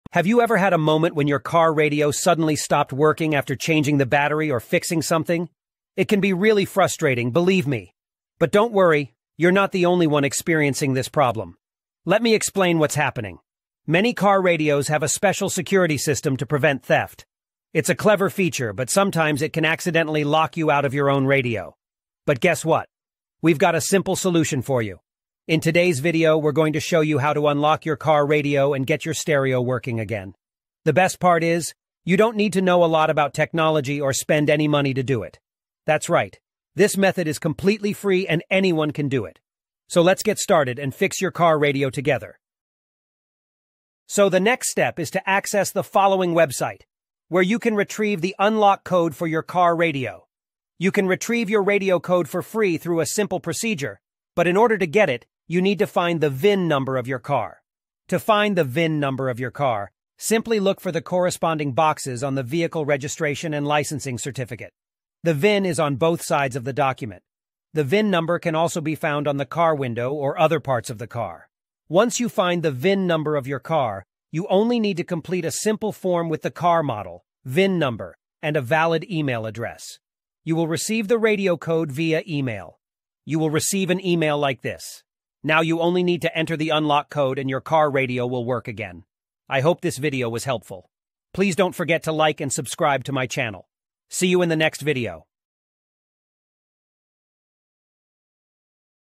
UNLOCK FORD RADIO How To Get Ford Radio Code FREE
Are you looking to unlock your Ford radio for free? You're in the right place. In this video, we'll show you a step-by-step process to obtain your Ford radio code without any cost involved. We understand how important it is to have a functional audio system in your vehicle, and we're committed to helping you unlock it without spending a dime.

We'll begin by explaining why radio codes are necessary and why they can sometimes become inaccessible. Understanding the background is essential for finding a solution.

Next, we'll provide you with specific, easy-to-follow steps to unlock your Ford radio for free. Our instructions are designed to be clear and user-friendly, ensuring a hassle-free experience.

Throughout the video, we'll share valuable tips and insights to make the unlocking process even smoother. We're dedicated to simplifying the solution for you.

So, whether you're a Ford owner or someone facing a locked radio, this video is your guide to unlocking it for free. Don't let a locked radio limit your driving enjoyment.